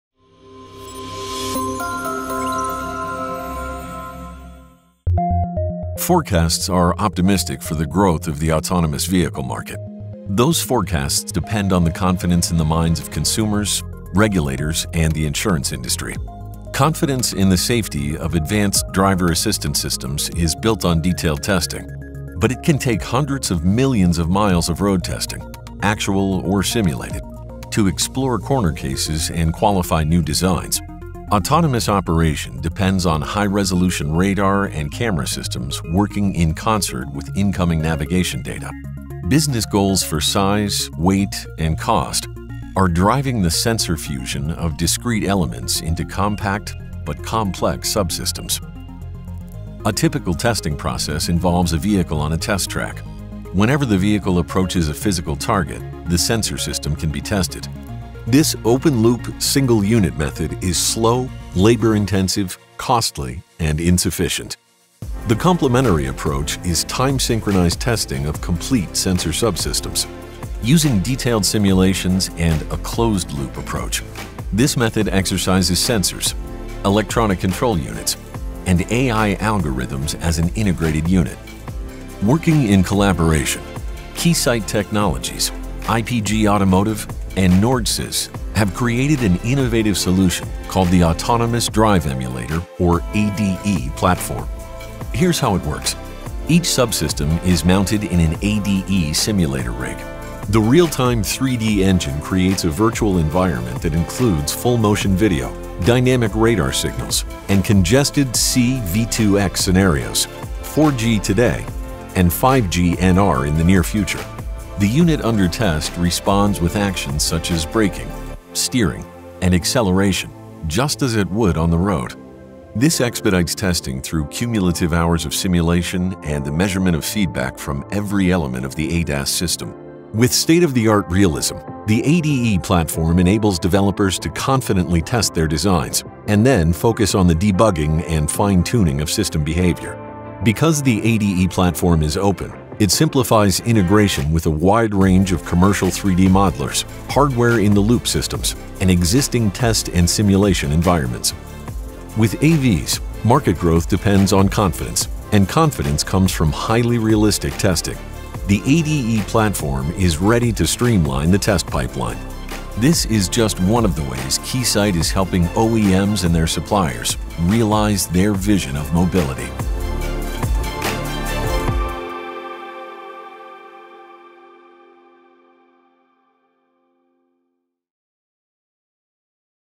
Build Confidence in ADAS Designs with Keysight’s Autonomous Drive Emulation (ADE) Platform
Accelerate testing of your advanced driver-assistance systems.

In the development of autonomous vehicles, confidence in the safety of advanced driver-assistance systems (ADAS) is built on detailed testing. This requires hundreds of millions of miles of road testing—actual or simulated—focused on exploring corner cases and qualifying new designs.

Keysight’s innovative Autonomous Drive Emulator (ADE) platform accelerates that process using detailed simulations and time-synchronized testing of sensors, wireless links, ECUs, and AI algorithms. With state-of-the-art realism, the ADE platform enables developers to thoroughly test their designs and then focus on the debugging and fine-tuning of system behavior.

This solution is just one of the ways Keysight and its technology partners are helping automotive OEMs and their suppliers realize their vision of mobility.